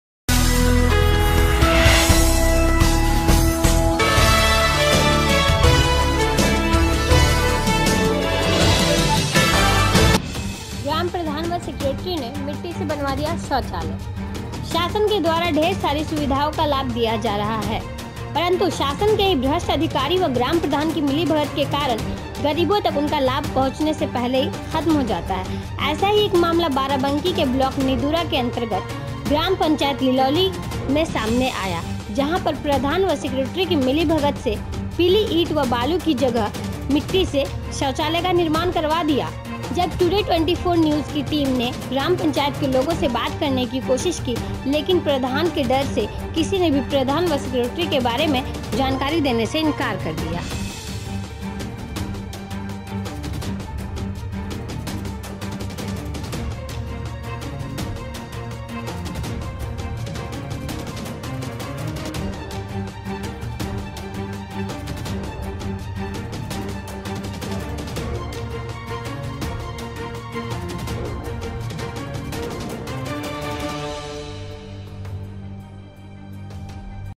ग्राम प्रधान वा सीक्रेटरी ने मिट्टी से बनवा दिया शौचालय-Today24
शासन के द्वारा ढेर सारी सुविधाओं का लाभ दिया जा रहा है परंतु शासन के ही भ्रष्ट अधिकारी वा ग्राम प्रधान की मिलीभगत के कारण गरीबों तक उनका लाभ पहुंचने से पहले ही खत्म हो जाता है ऐसा ही एक मामला  बाराबंकी के ब्लाक निदूरा के अंतर्गत ग्राम पंचायत लिलौली में सामने आया जहाँ पर प्रधान वा सीक्रेटरी की मिली  भगत से पीली ईट

T.V. News

Application.

views!!!

News."